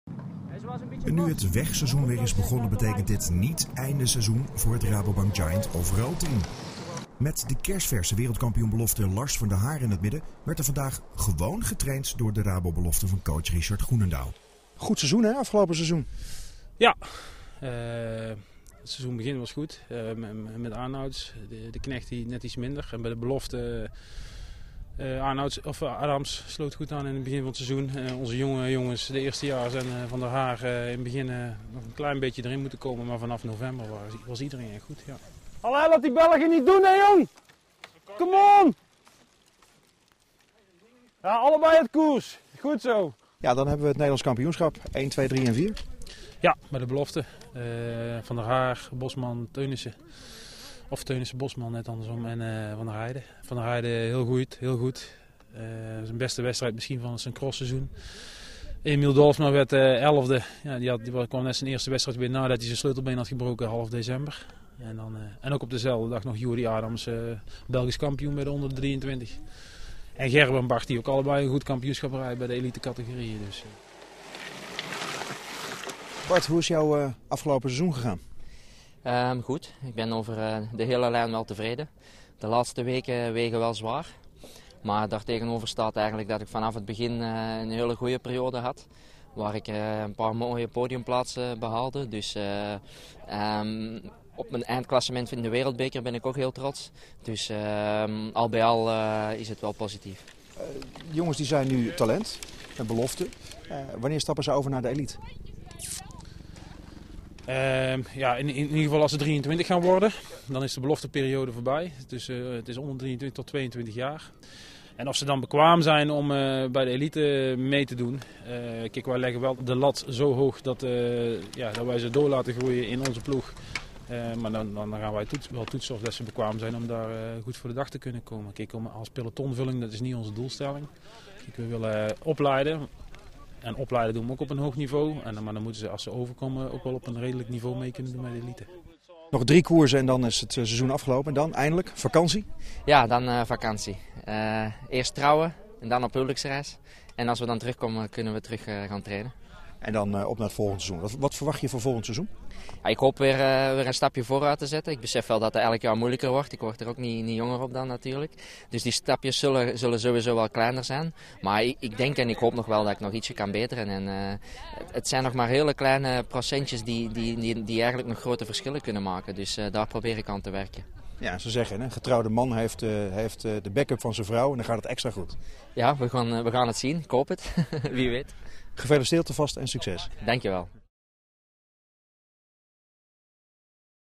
Training Offroad Team
Het seizoen loopt op zijn eind. We blikken terug met ploegleider Richard Groenendaal tijdens een training in Alphen.